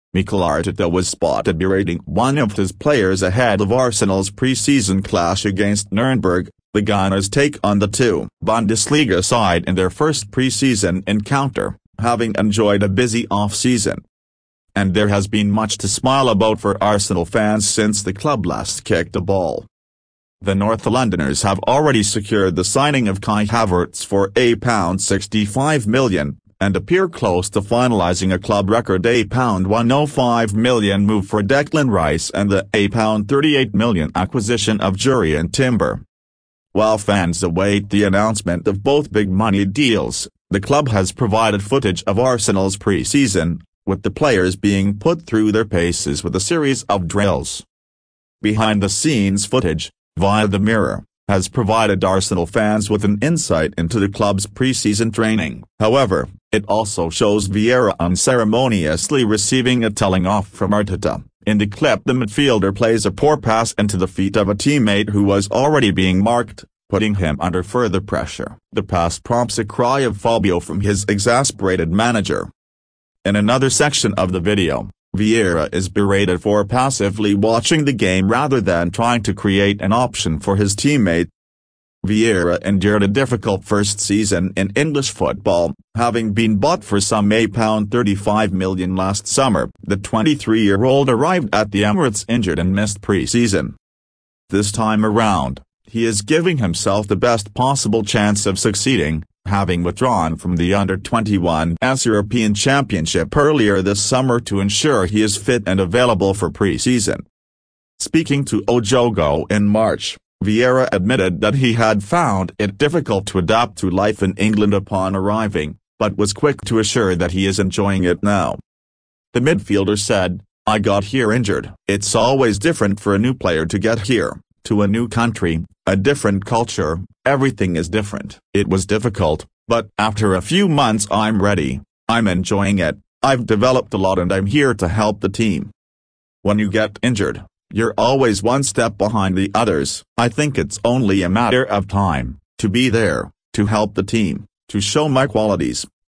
Mikel Arteta Spotted Berating Arsenal Star Fabio Vieira in Furious Training Ground Incident.
Mikel Arteta was spotted berating one of his players ahead of Arsenal’s pre-season clash against Nurnberg.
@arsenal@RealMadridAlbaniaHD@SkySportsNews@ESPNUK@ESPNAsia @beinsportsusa